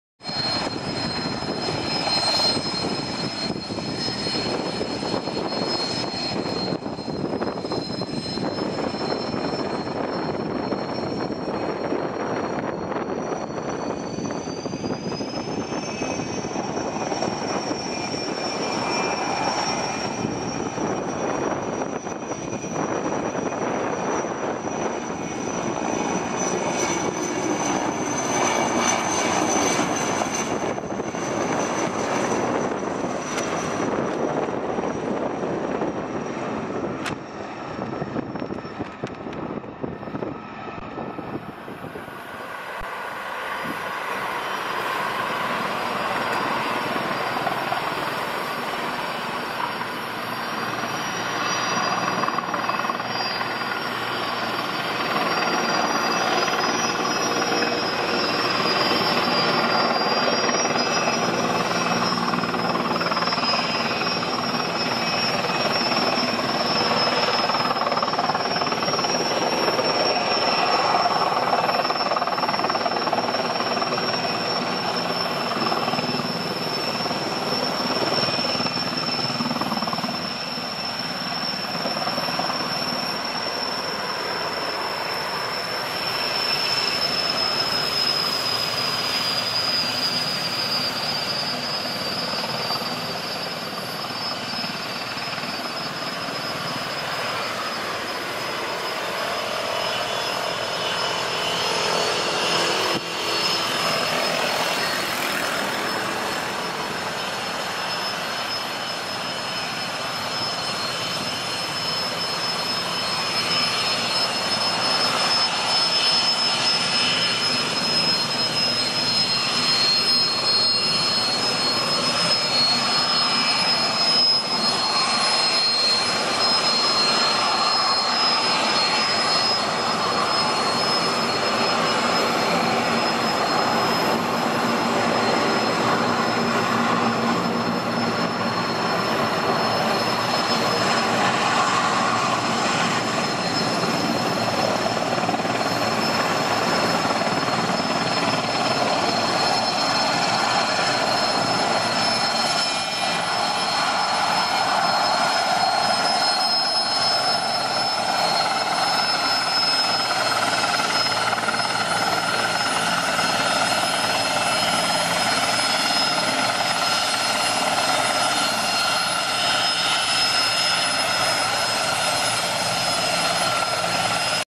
Erickson Air-Crane S64E N154AC Georgia Peach.
Erickson Air-Crane Sikorsky S64E N154AC Skycrane (MSN 64-037), named ‘Georgia Peach’ take off, landing and taxiing.